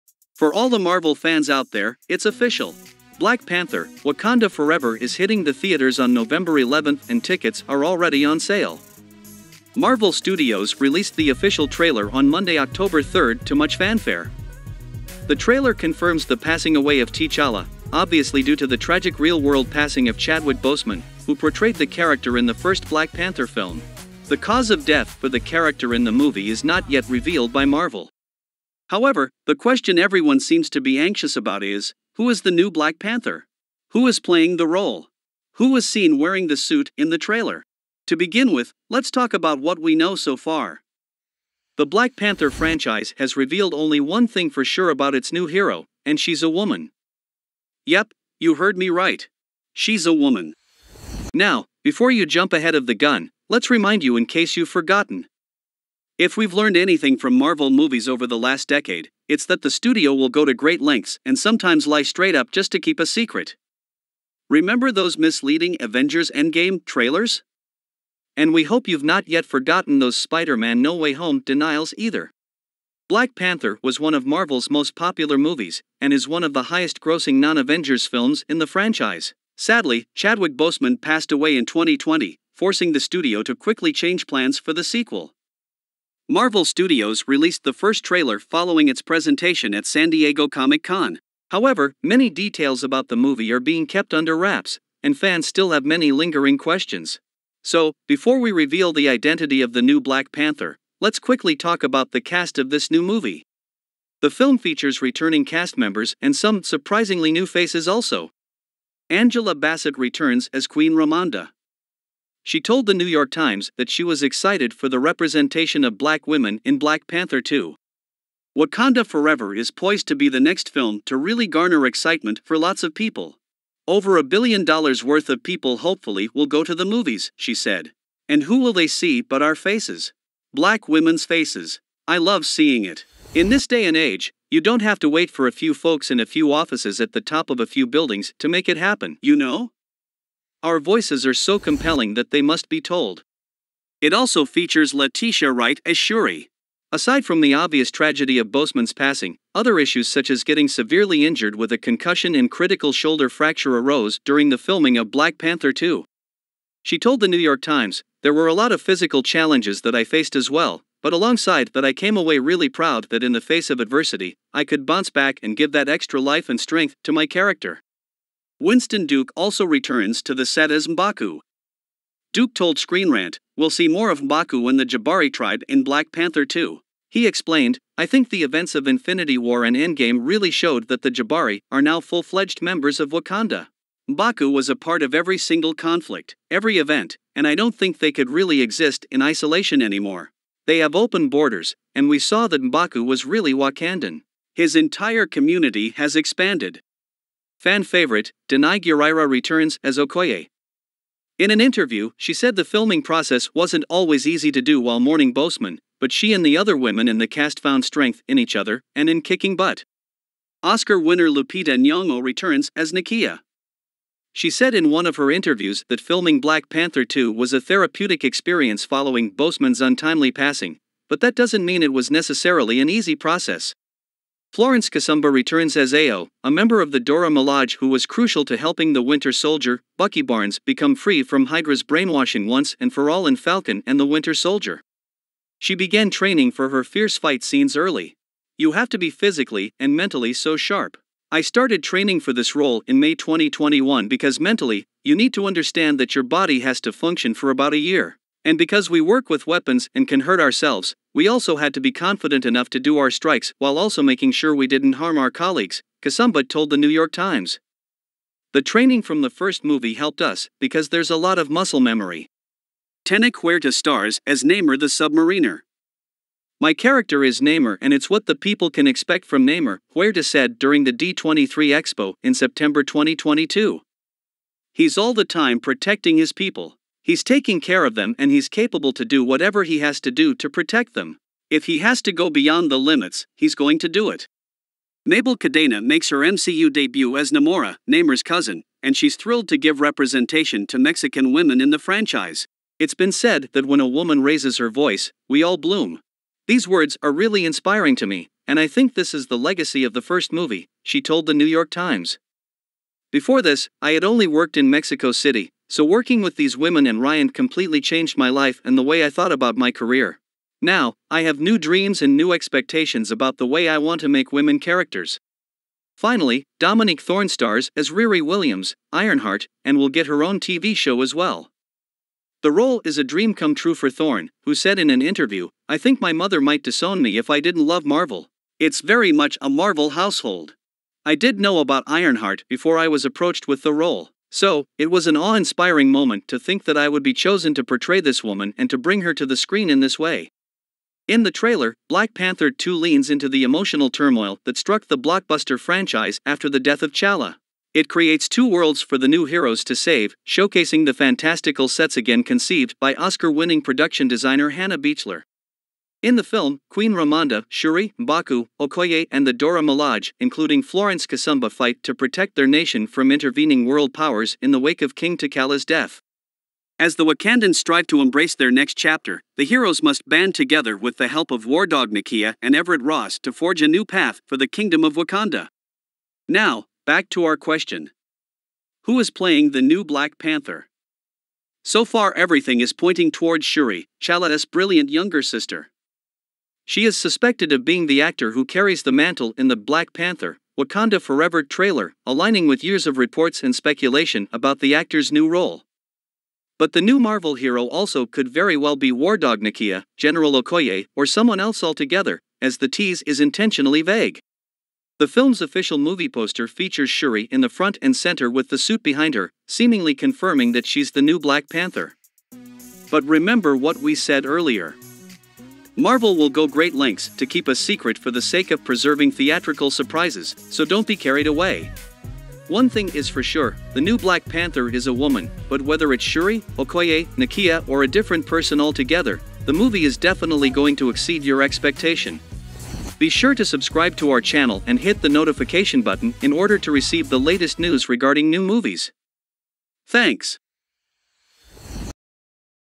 Black Panther 2: Who is the New Black Panther in the Wakanda Forever Trailer | Movie Cast Revealed
blackpanther was one of Marvel's most popular movies, and is one of the highest grossing non-Avengers films in the franchise. Sadly, Chadwick Boseman passed away in 2020, forcing the studio to quickly change plans for the sequel. Marvel Studios released the first trailer following its presentation at San Diego Comic Con.
However, many details about the moviereview are being kept under wraps, and fans still have many lingering questions. So, before we reveal the identity of the new Black Panther, lets quickly talk about the cast of this new movie.